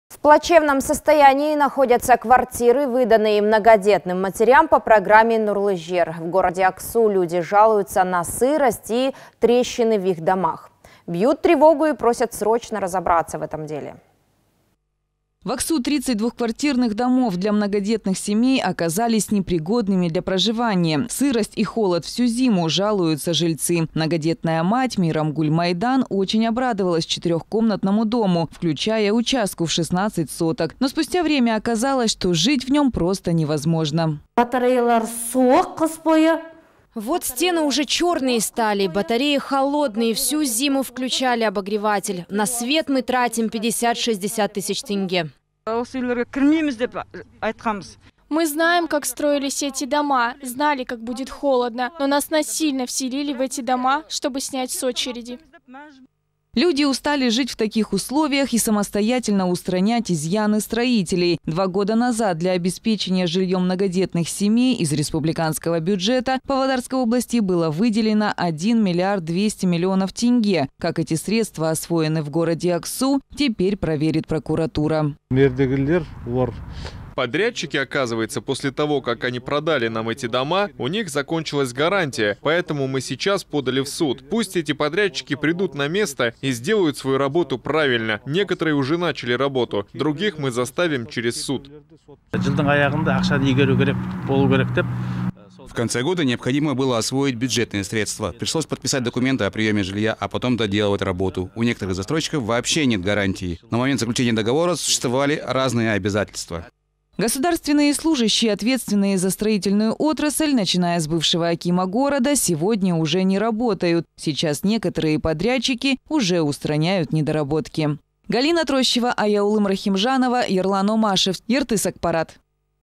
КВАРТИРЫ В ПРОБЛЕМНЫХ ДОМАХ ВЫДАЛИ МНОГОДЕТНЫМ МАТЕРЯМ
В плачевном состоянии находятся квартиры, выданные многодетным матерям по программе «Нурлы жер». В городе Аксу люди жалуются на сырость и трещины в их домах. Бьют тревогу и просят срочно разобраться в этом деле.